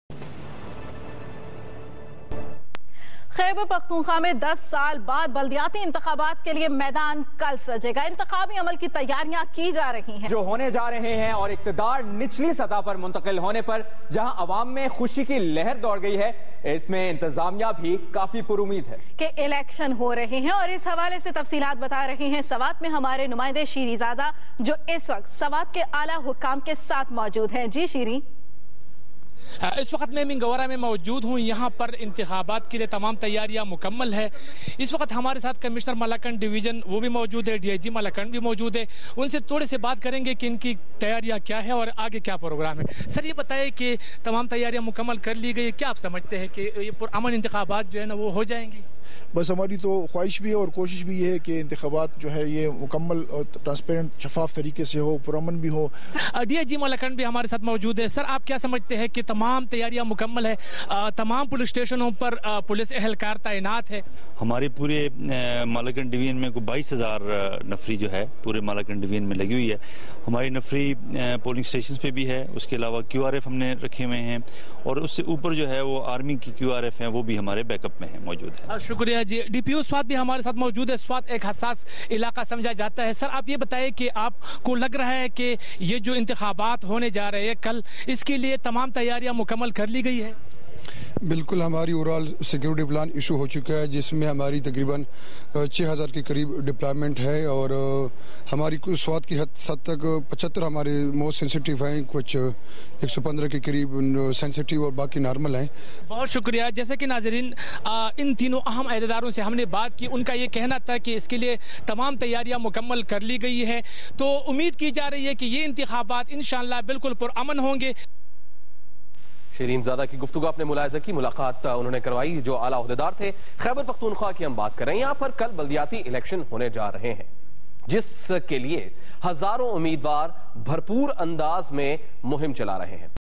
Local Body Election in swat with kamishnar malakand DIG and DPO swat valley
سوات میں بلدیا تی الیکشن کمیشنر ملا کنڈ ڈیثرن،ڈی ائ جی ملا کنڈ ڈیثراورڈی پی او سوات کا کیا کہنا ہے دیکھے ۔